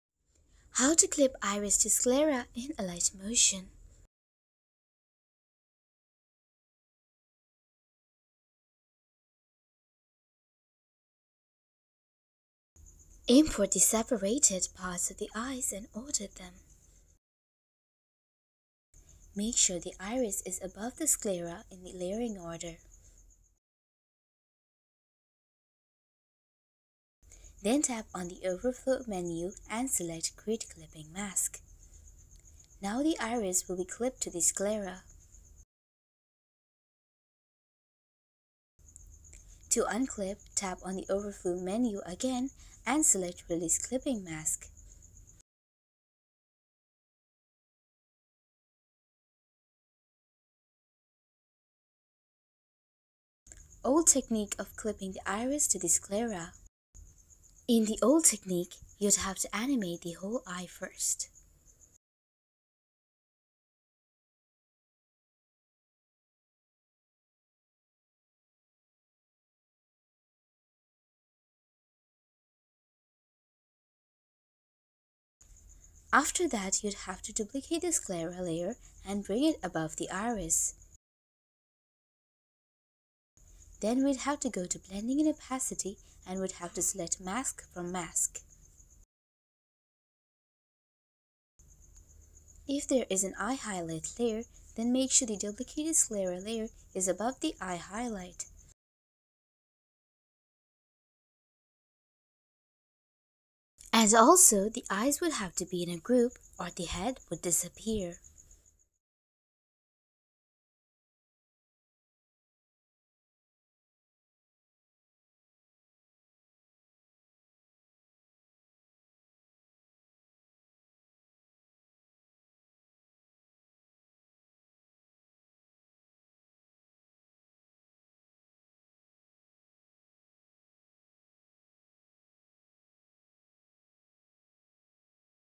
[UPDATED] How to CLIP the Iris to the Sclera in Alight Motion
✦ Chapters:
00:00 Intro
00:04 Demo
00:11 Updated Eye Clipping Technique
00:53 Old Eye Clipping Technique

✦ Alternate Titles:
How to make the Iris Stay within the Eyeball
How to move the iris Within the sclera
How to make the iris a subset of sclera